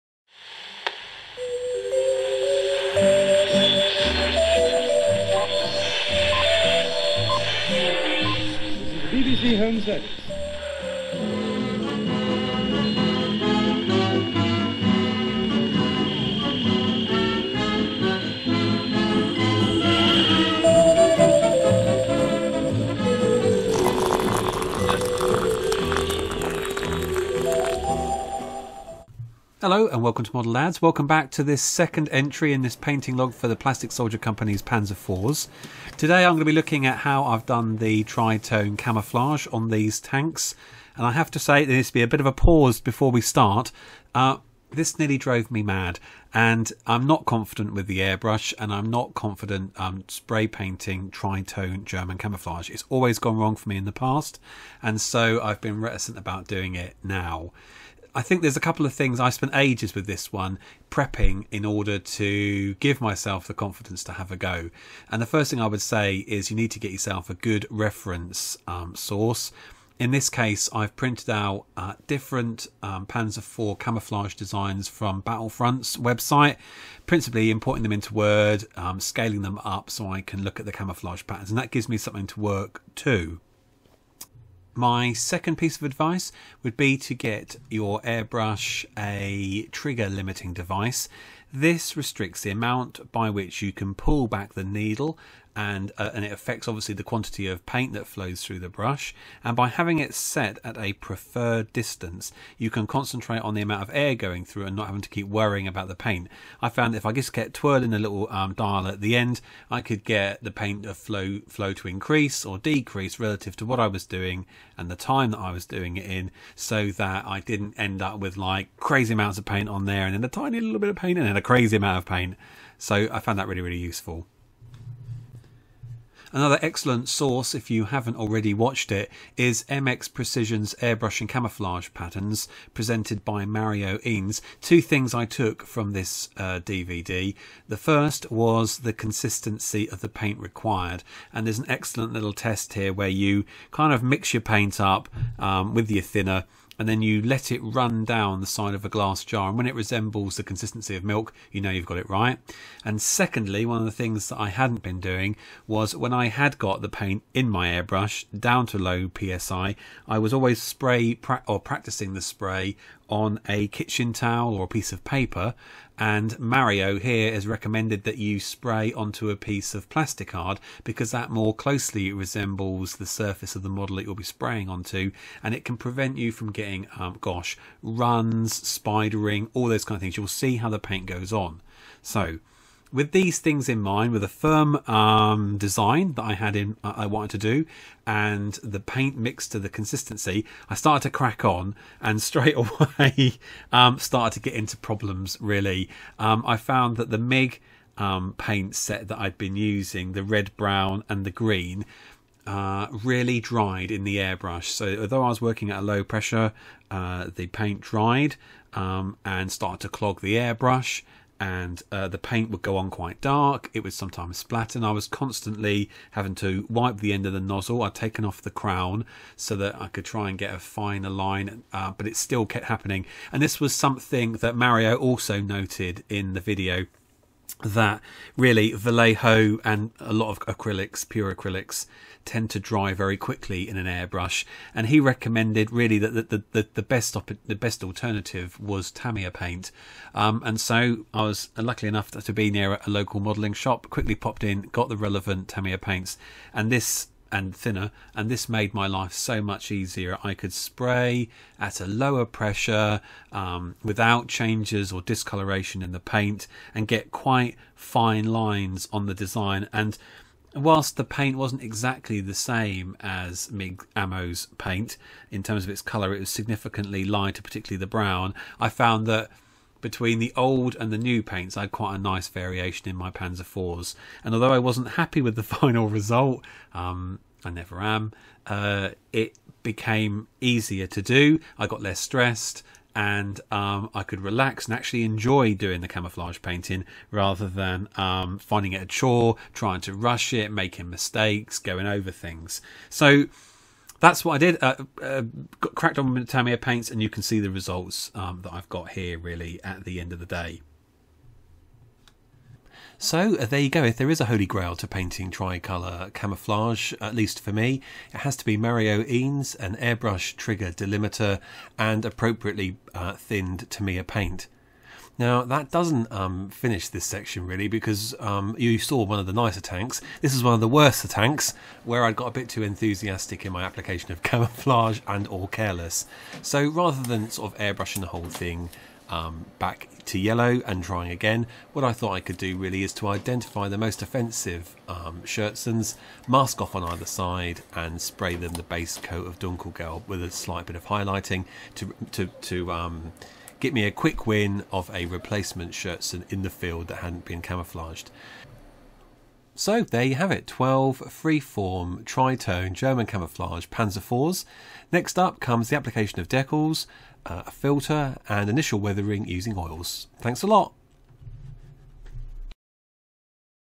15mm Panzer IV painting log: free-hand tri-tone camo
I risk multiple nervous breakdowns trying to obtain a convincing soft-edged free-hand tri-tone camouflage pattern on 12 tanks.

Unofrtunately, the MXpression DVD mention in the vlog is no longer in production but someone has thoughtfully uploaded a cheeky copy

Fill your boots :)